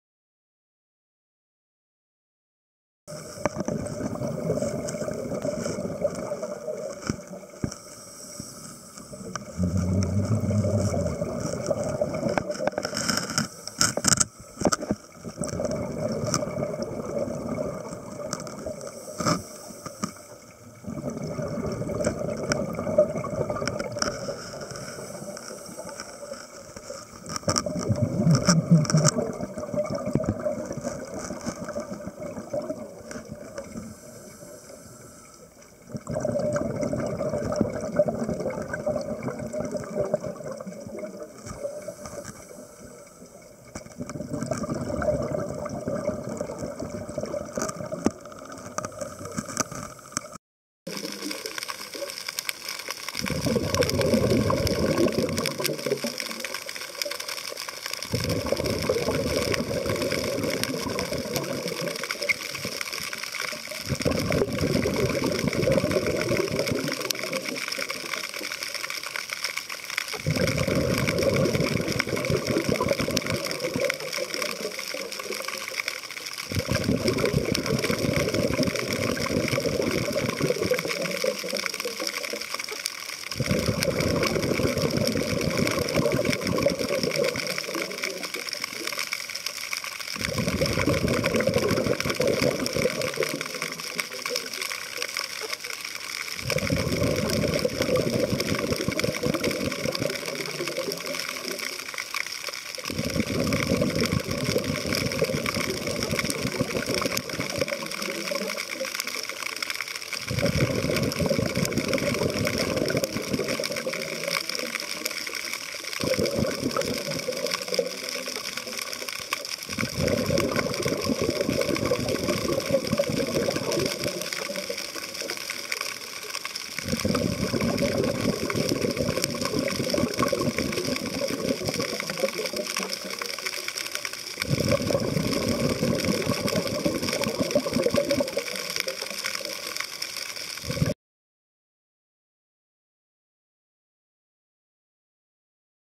Not SEA BUNNY,it's SEA DEVIL.
Melibe papillosa is sea slug.
location:Izu Peninsula JPN.